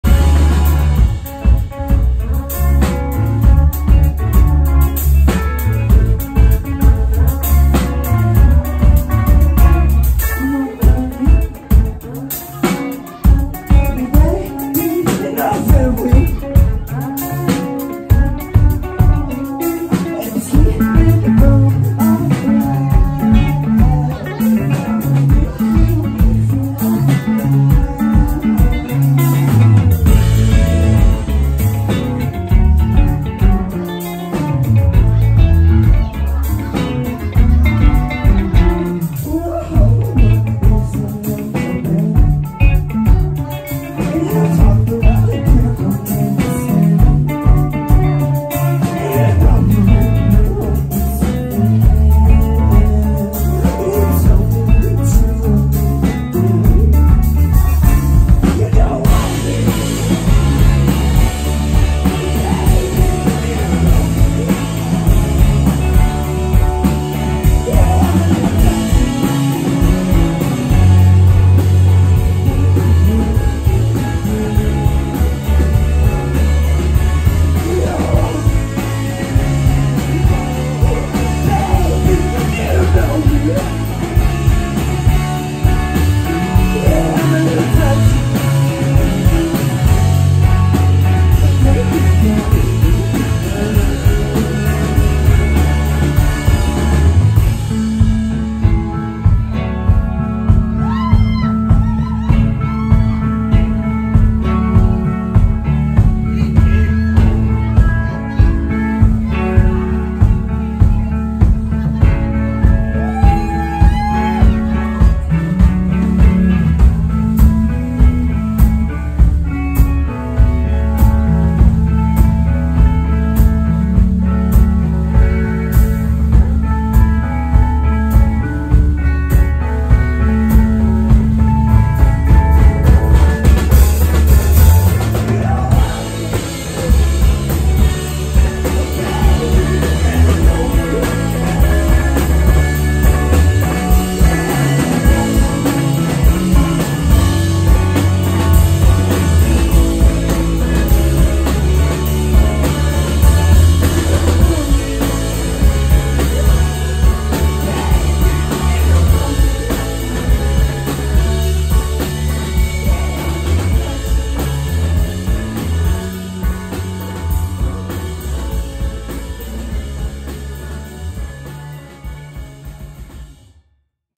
Audio Book Club
What can I say? A local favorite rock in roll band. Gotta catch em live.